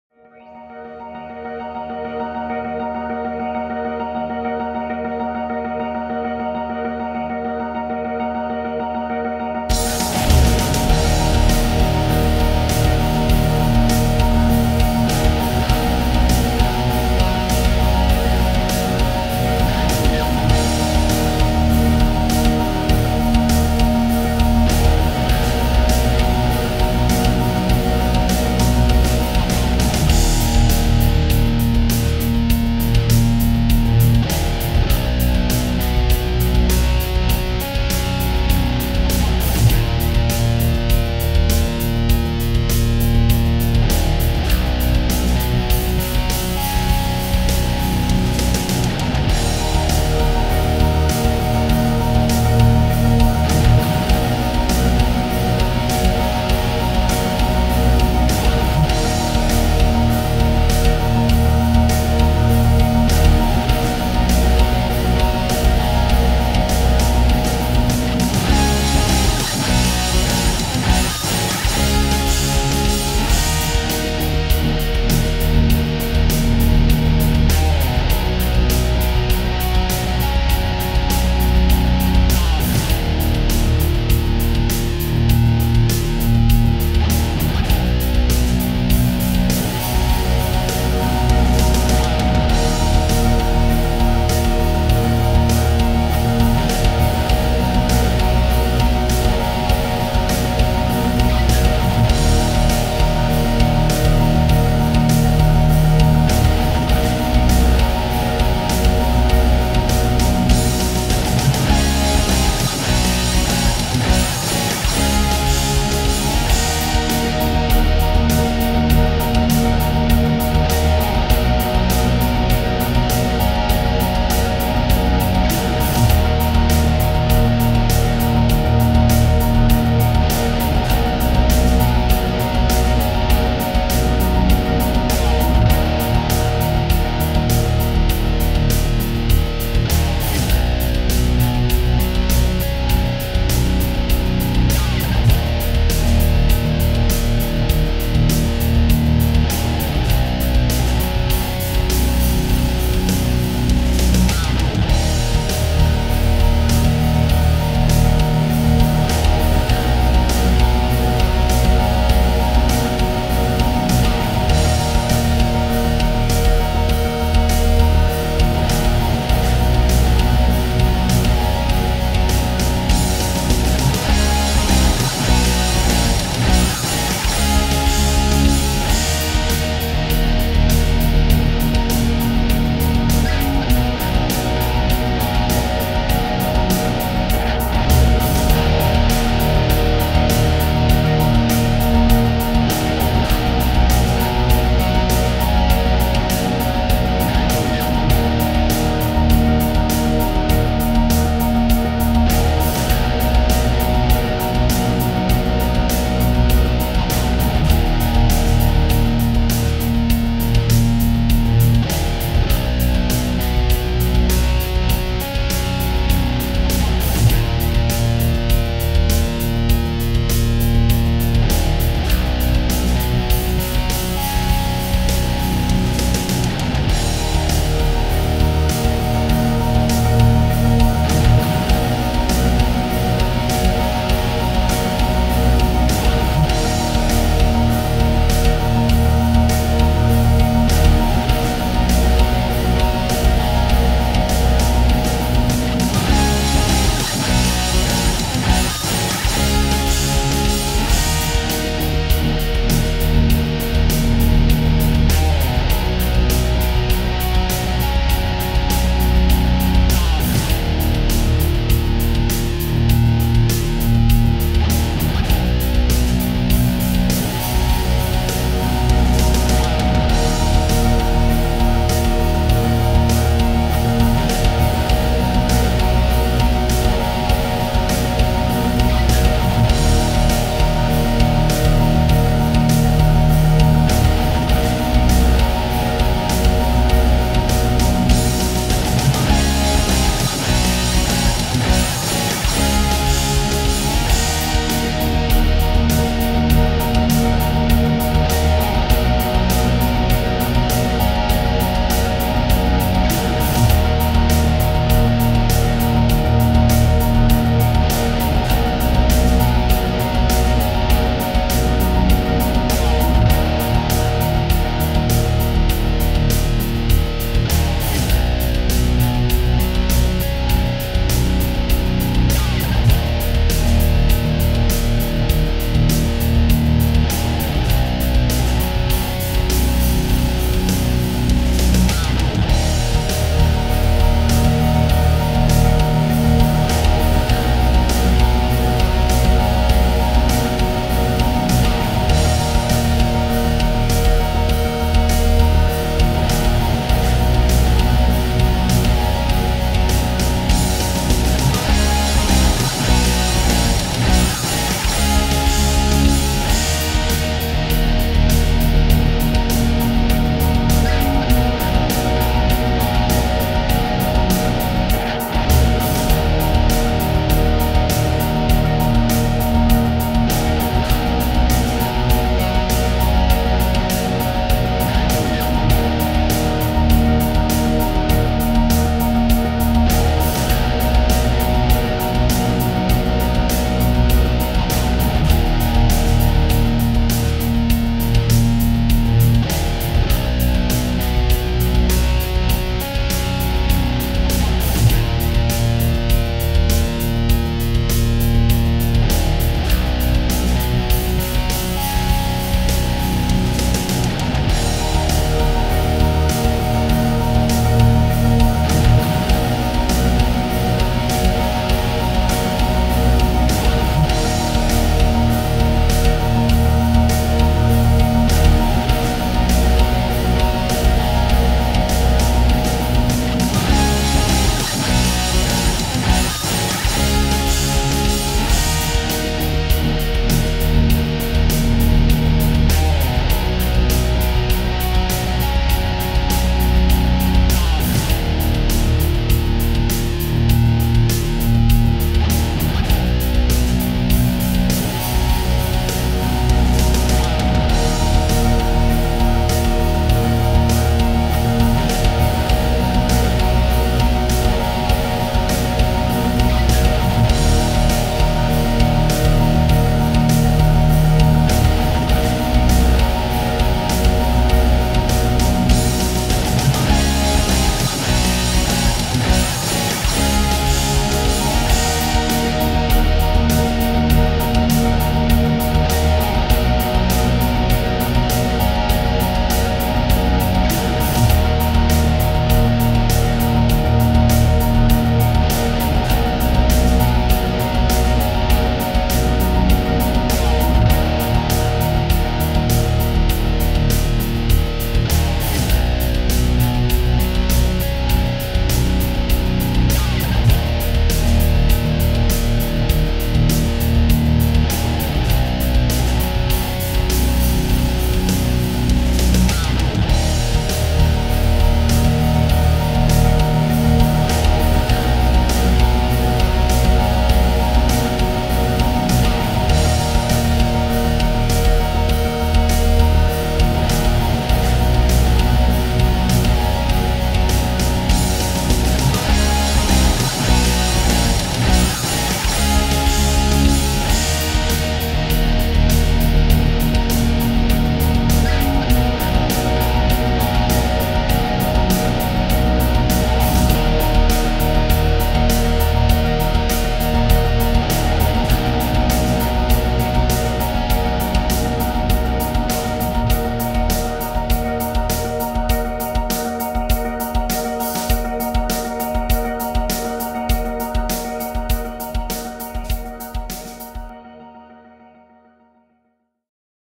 D# Minor Epic Metal Guitar 7-String Backing Track
Chords:
D#min to Bmaj for the majority of the song. I would D# minor scale or pentatonic to improvise and write melodies. The hook is C# Maj-G#Maj, Bmaj-F#Maj, Bmaj-F#Maj-C#Maj, D#min.